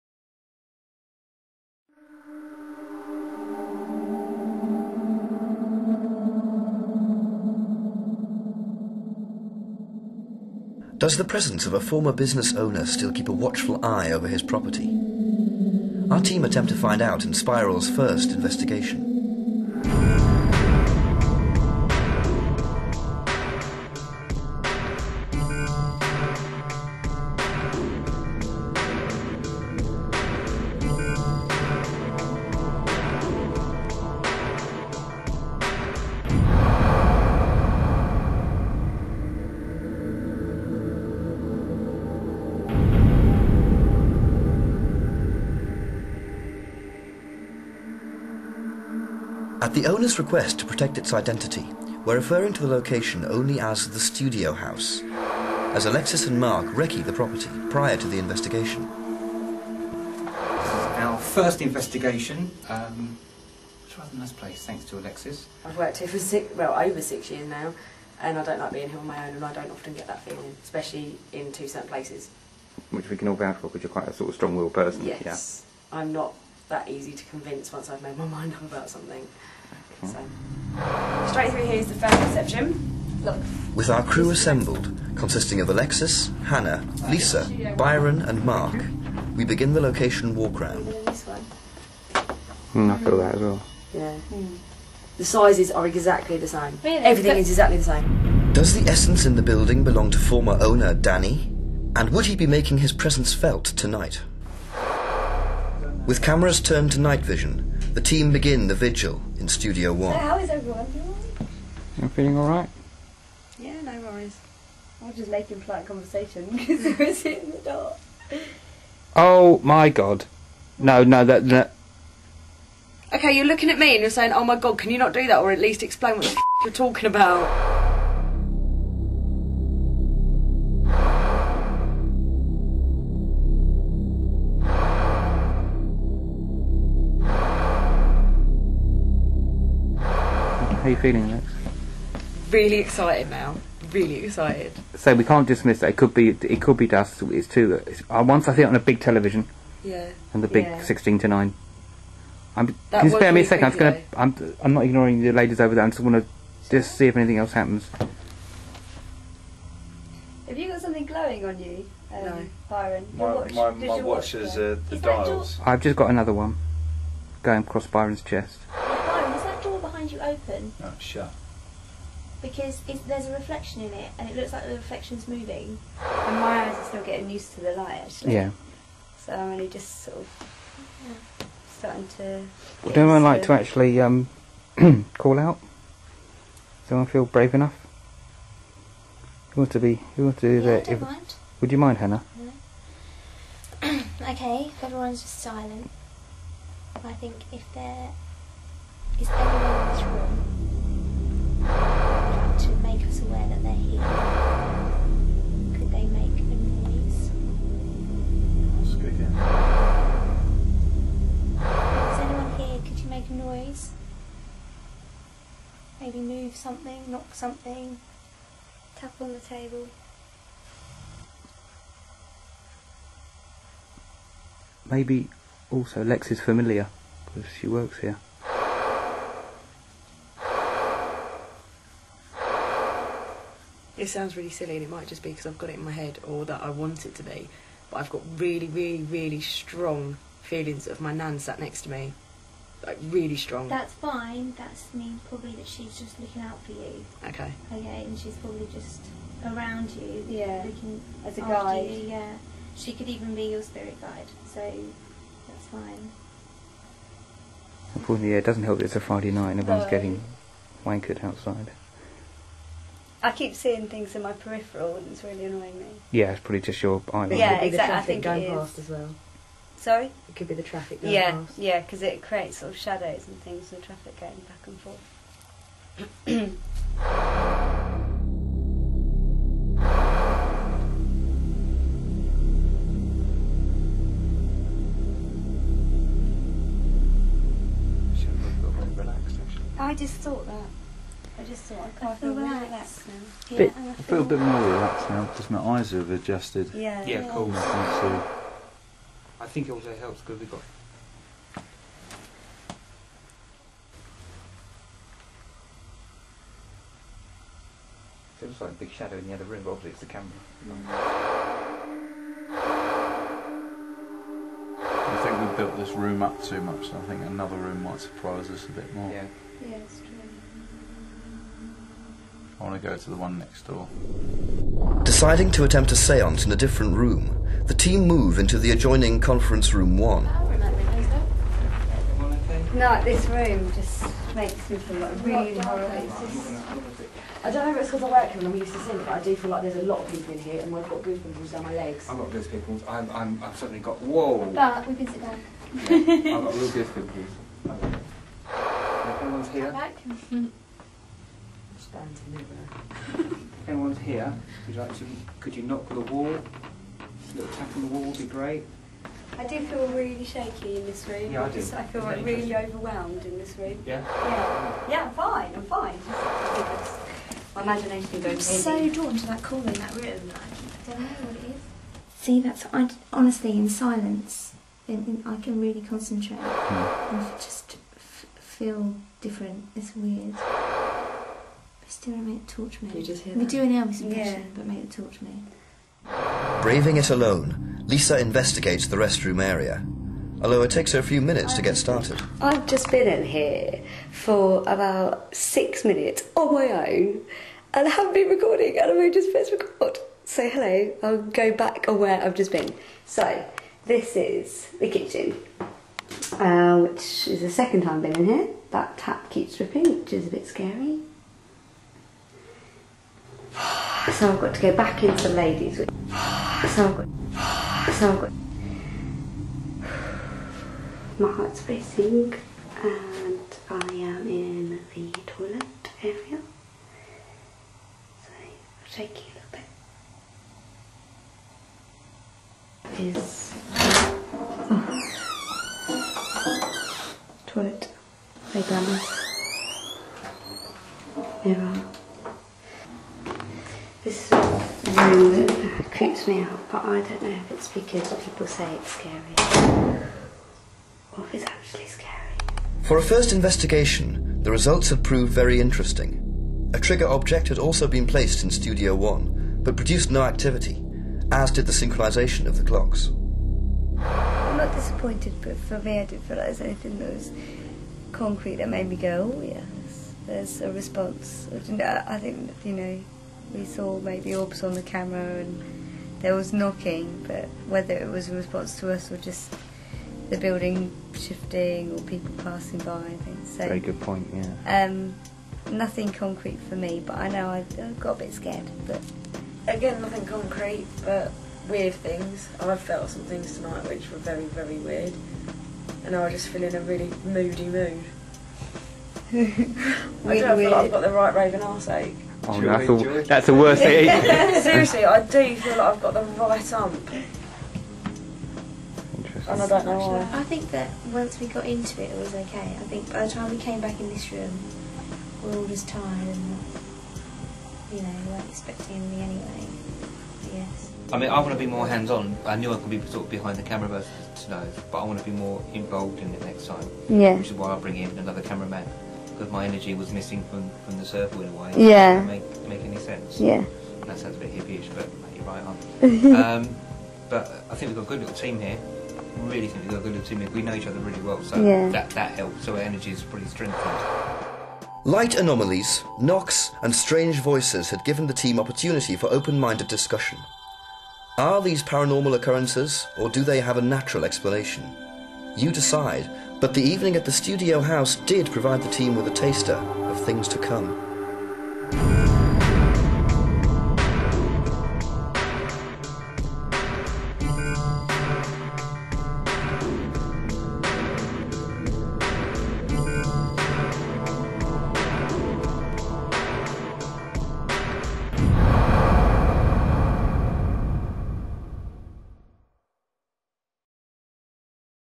Spiral Episode 1 - The Studio House
Spiral's first investigation and the first online episode. Numerous reports of an unseen presence in this refurbished building bring the team to explore the location which provides a taster of things to come.

This is an edited version of a higher quality video Episode which is available